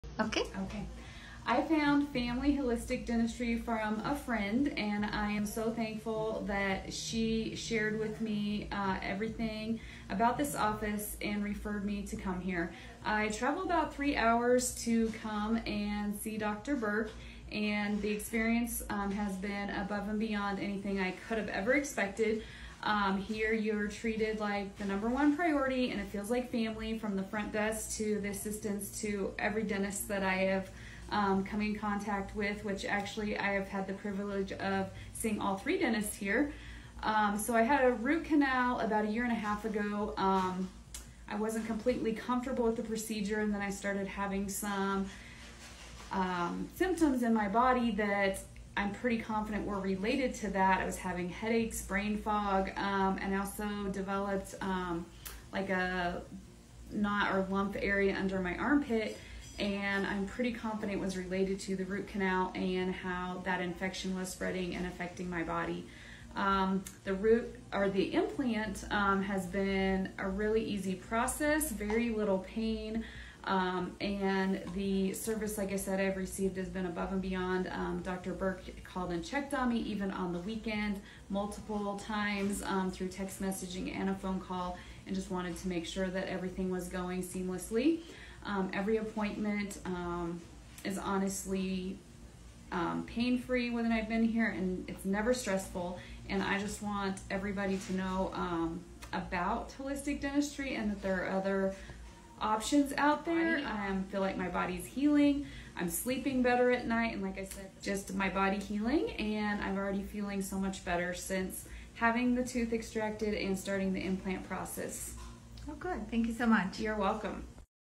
Testimonial about a CeraRoot implant ( ceramic/zirconia implant)placed at Family Holistic Dentistry
TESTIMONIAL ABOUT A CERAROOT IMPLANT 😊our patient shares with us how much she likes our office familyholisticdentistry because the customer service is above and beyond she has never expected. In addition, she shares how a failed root canal treated tooth wasn’t feeling right and the infection was spreading .
She feels much better since the tooth was extracted and the ceraroot implant was placed. She was 2 months from the placement date.
She is sleeping better since the procedure was done .